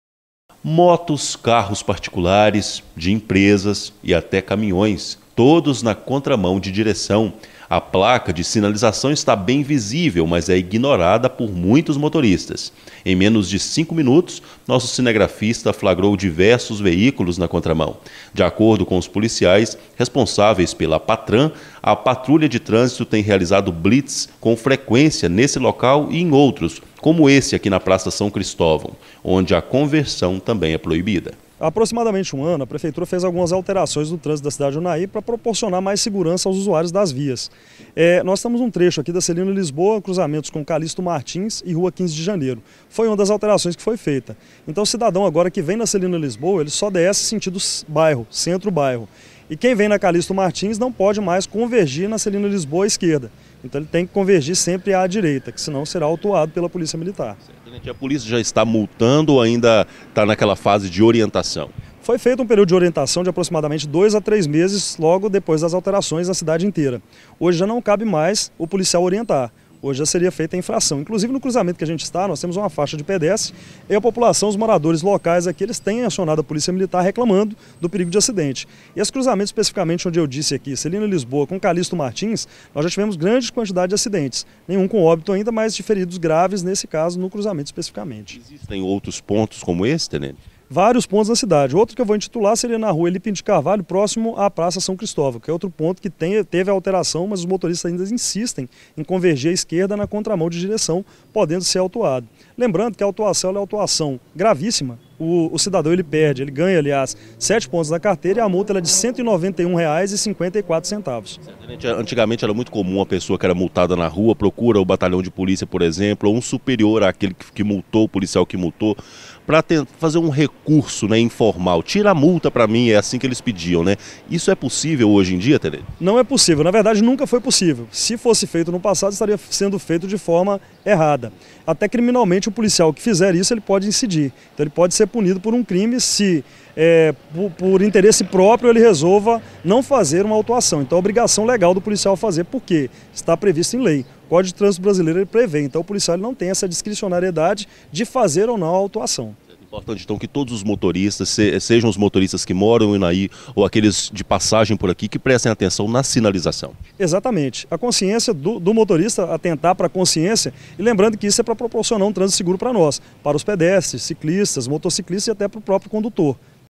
Vários Veículos são multados pela Patrulha de Transito em Unaí
Algumas ruas da cidade de unai tem causado polêmica entre os motoristas. Acontece que de um tempo para cá a direção e a sinalização de algumas ruas foram alteradas  mas muita gente insiste em transitar na contramão. Confira.